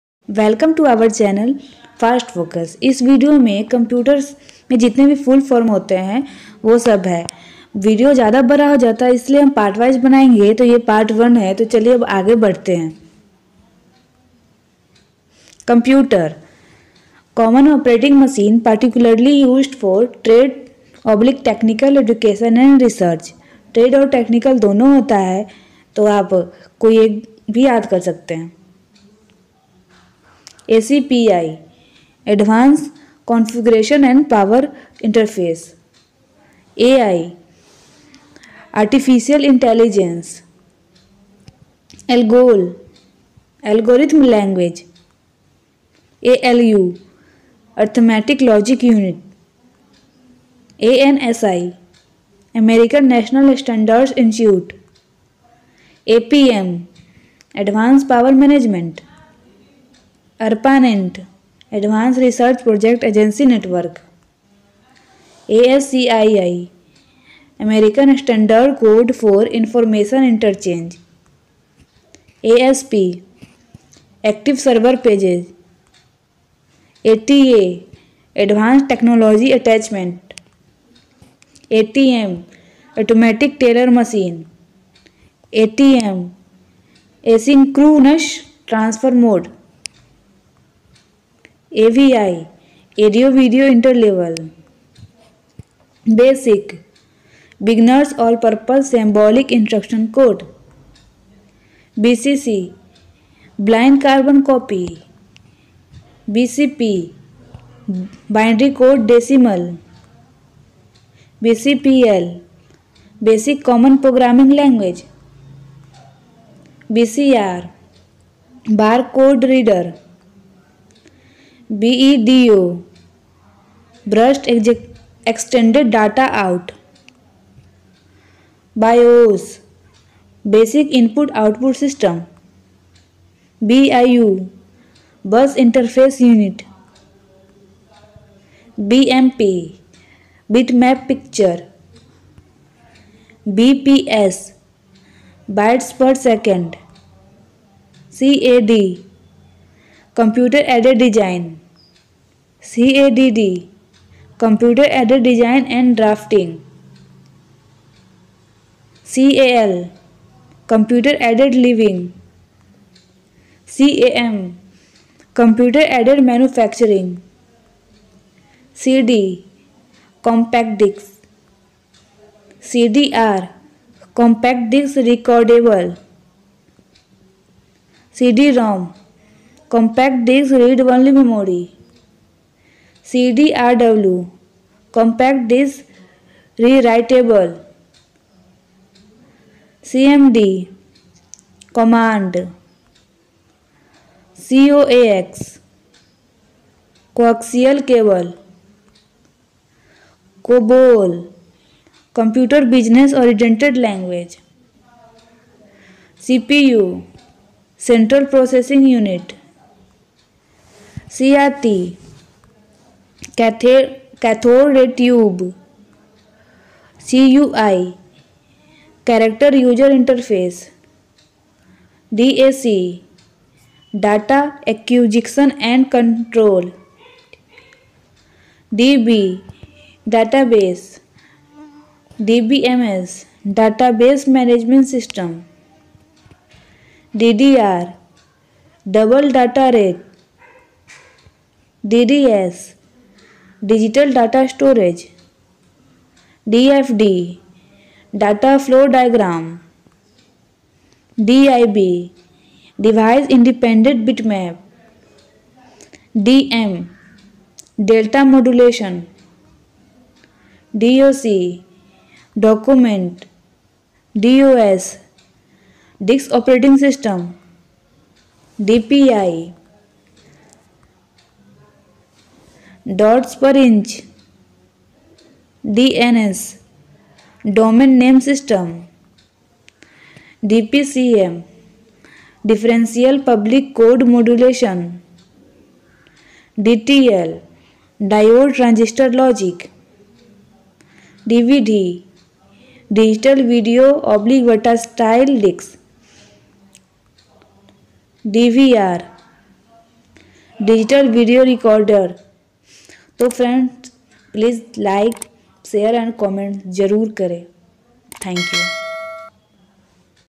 Full Form of Computer
This video contains about the full form of computer , all full form of computer.

If you enjoy this, give you ‘Like’ and ‘ Share ’ this video as much as possible. Don't forget to write your feedback in the “Comment Box”.